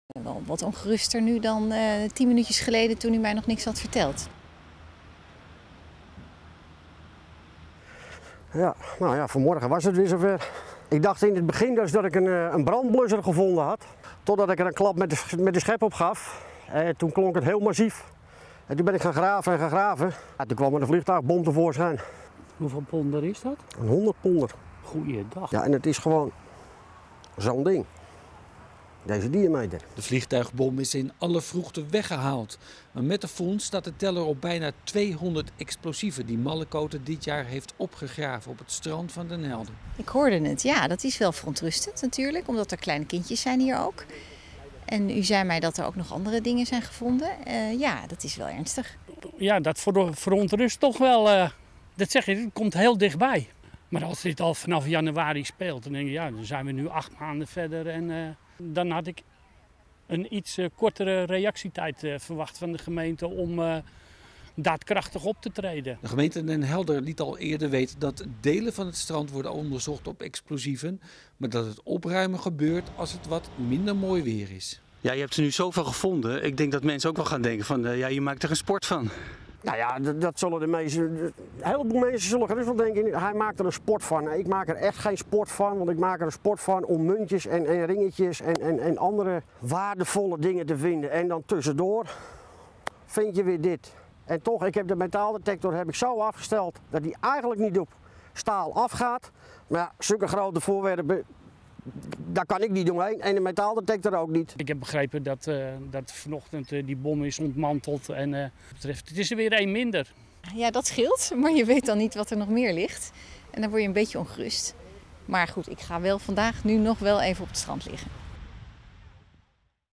Opnieuw vliegtuigbom gevonden op strand Den Helder
Op het strand van Den Helder is een vliegtuigbom uit de Tweede Wereldoorlog gevonden. Strandzoeker Ed Mallekoote trof de 100-ponder aan vlakbij een strandpaviljoen. De bom is in alle vroegte onschadelijk gemaakt door de Explosieven Opruimingsdienst Defensie (EOD).